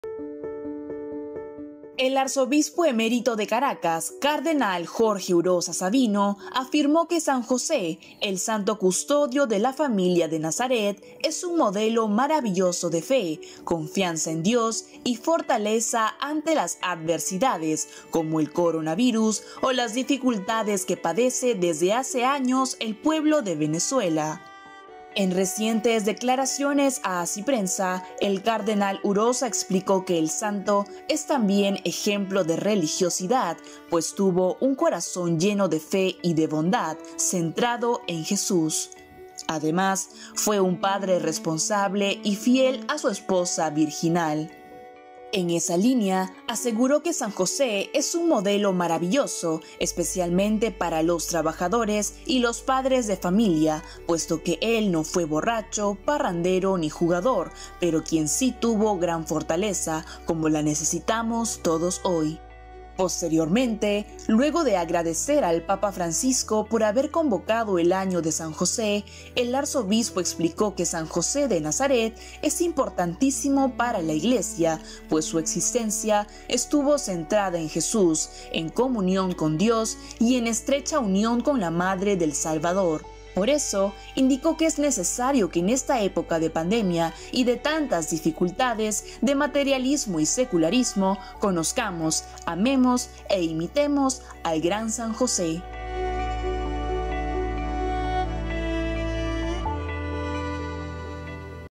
Cardenal afirma que San José es modelo maravilloso de fe
En Venezuela, el Arzobispo Emérito de Caracas afirmó que San José es un modelo maravilloso de fe y confianza en Dios ante las adversidades. Veamos.